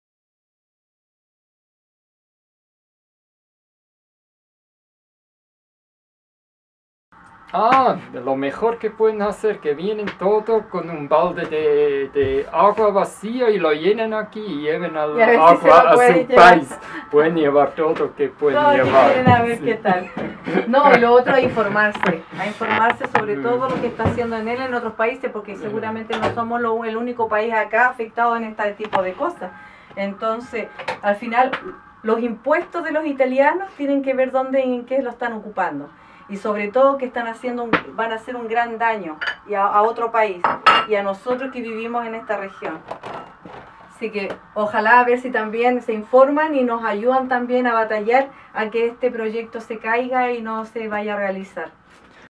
Messaggio di Ninoska e Werner - STOP ENEL (sottotitoli ITA)
Un messaggio agli italiani di Ninoska e Werner della Agrupaciòn del Rio Murta.
traduzione Elsa C.